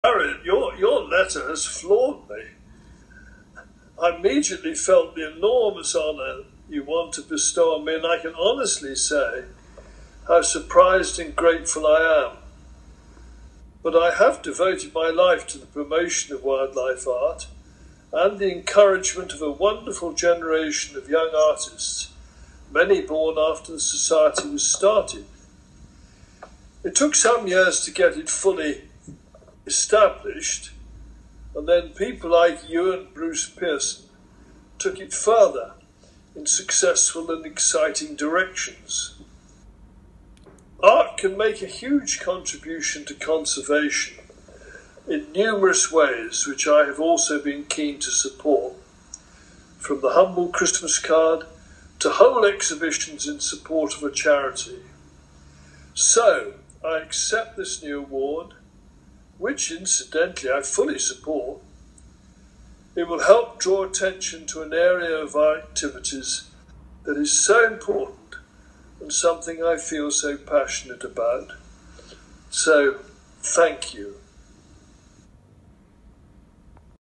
Robert Gillmor accepting the SWLA Outstanding Contribution to Art & Nature Award
Robert Gillmor MBE, Founder of the SWLA and Past President accepts the SWLA Outstanding Contribution to Art & Nature Award in recognition of his huge contribution to the SWLA, other conservation charities and wildlife art throughout his lifetime.